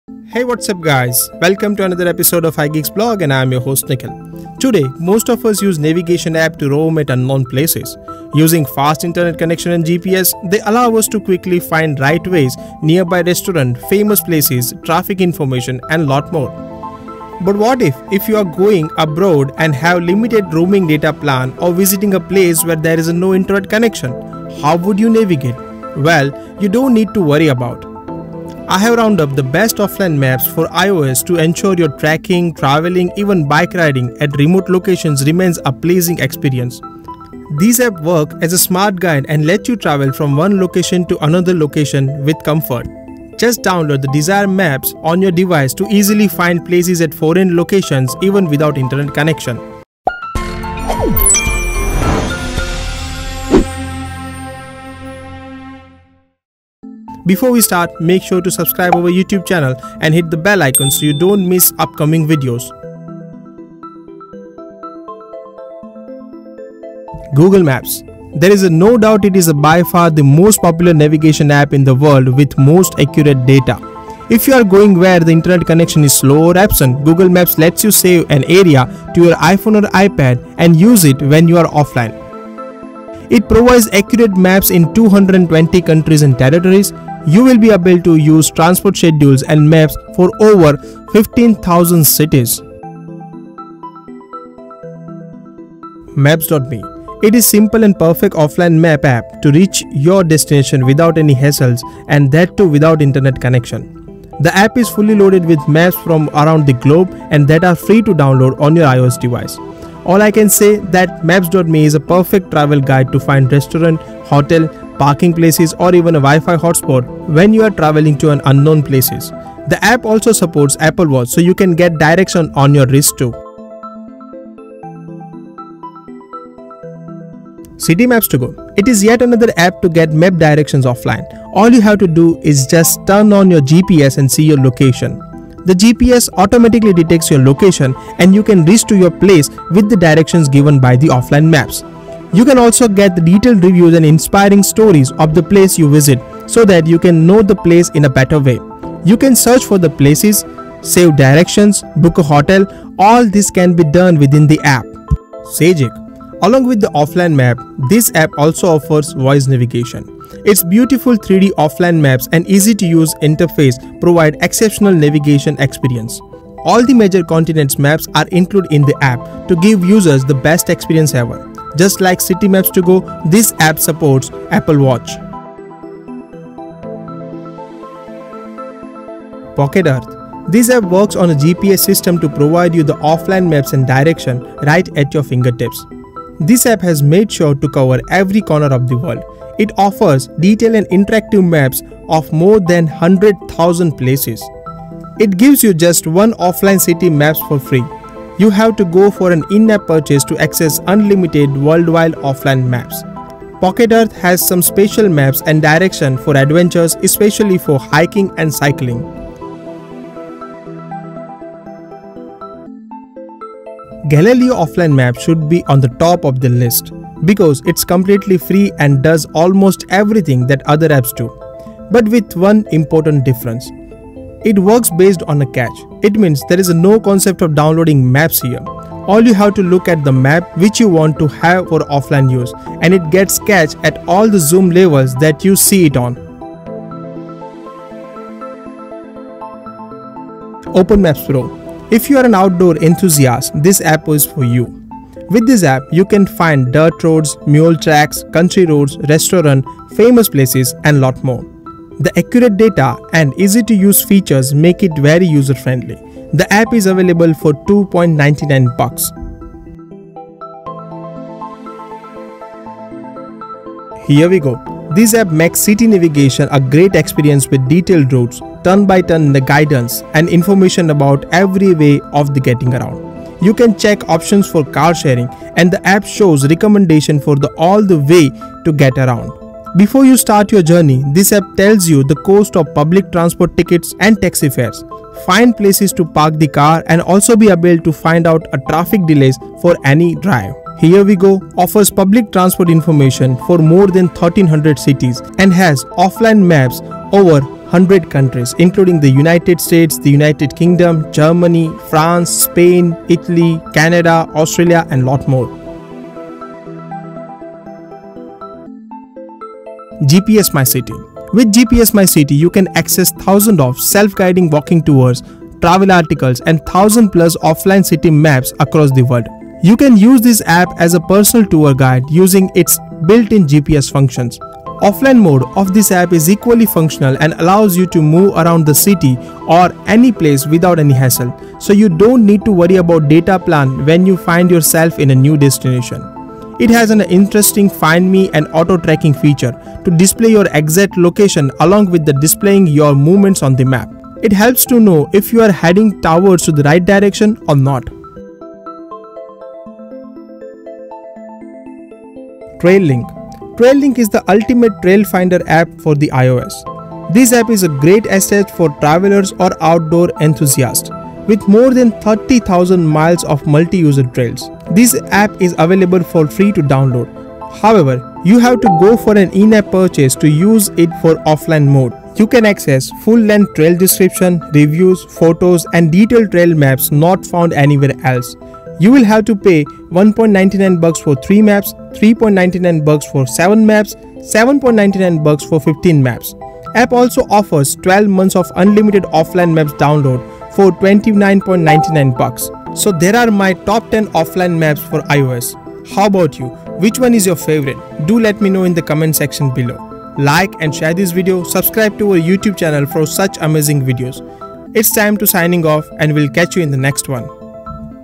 10 Best Offline Map Apps for iPhone and iPad (2018)
In this video, I round up top 10 best offline Map apps for iPhone and iPad. These Map apps let you comfortably navigate at unknown places even without Internet connection.

1. Google Maps (Free)

2. Maps.me (Free)

3. CityMaps2Go Offline Map (Free)

4. Sygic (Free)

5. Pocket Earth (Free)

6. Galileo Offline Maps (Free)

7. OpenMaps PRO ($2.99)

8. HERE WeGo (Free)

9. GPSmyCity (Free)

10. TrailLink (Free)